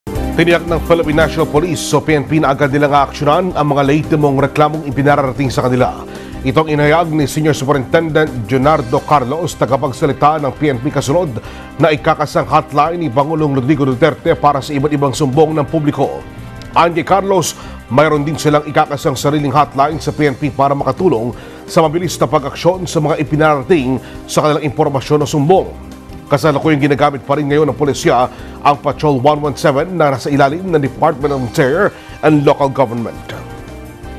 PNP may ikakasang hotline para mapabilis ang pag-aksyon
MANILA, Philippines (Eagle News) -- Tiniyak ng Philippine National Police o PNP na agad nilang aaksyunan ang mga lehitimong reklamong ipinararating sa kanila.